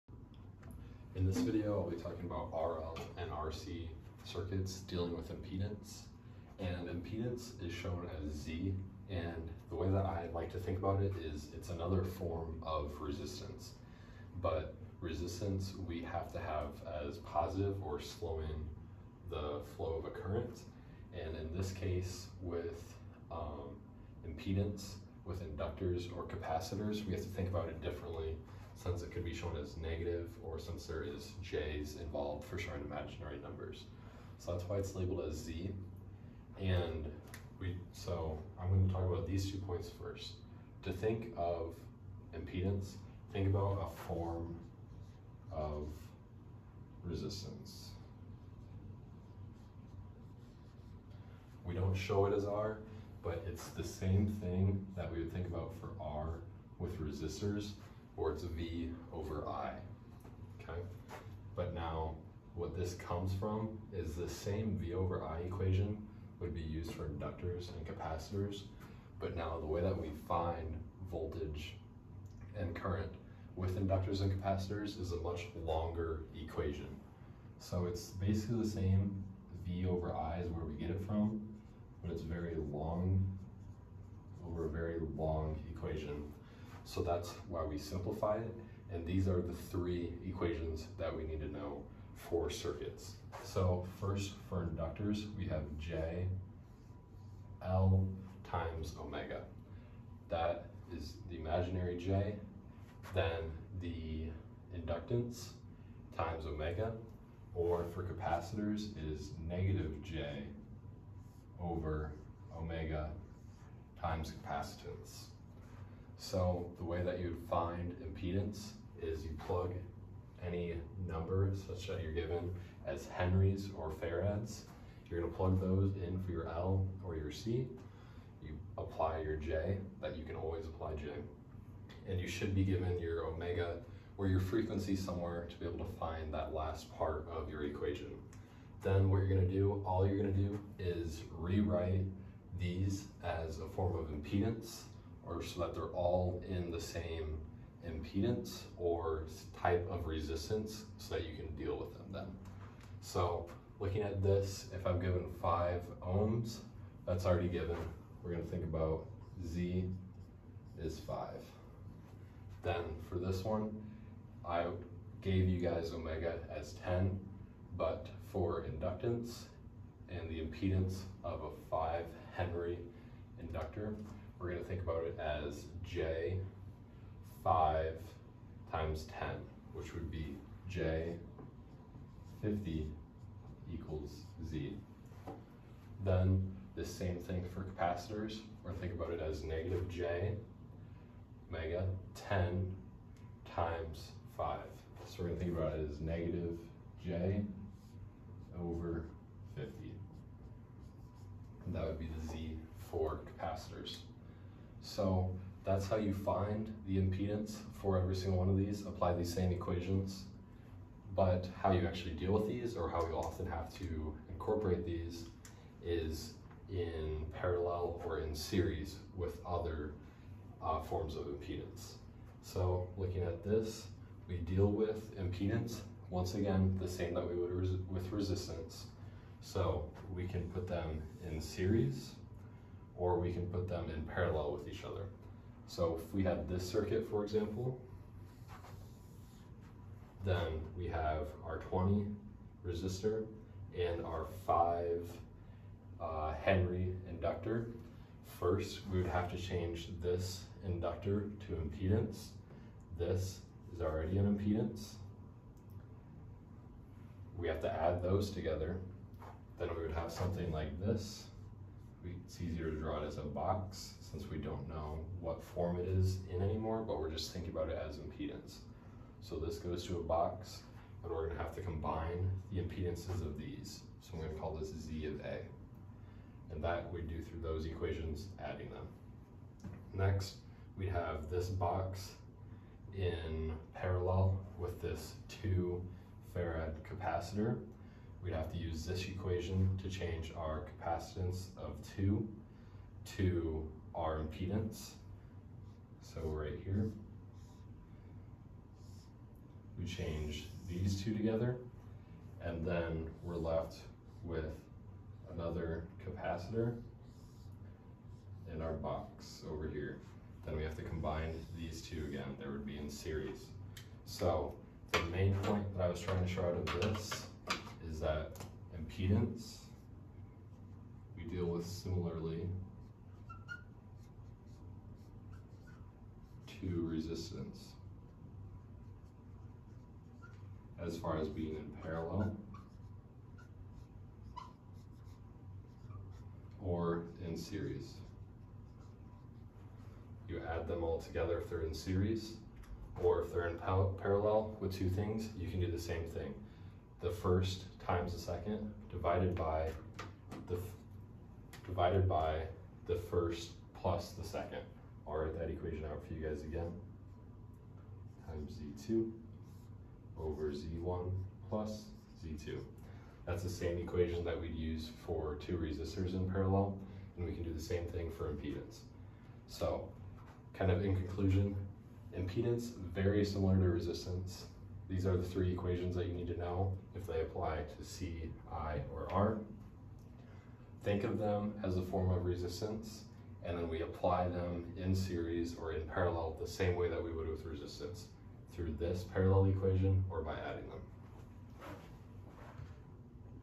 2.4/2.5 - RL and RC Circuits - determining L and C or equivalent impedance
Equations for impedances from resistors, capacitors, inductors
Similarities to resistance
Where equations are from
How to use in series or parallels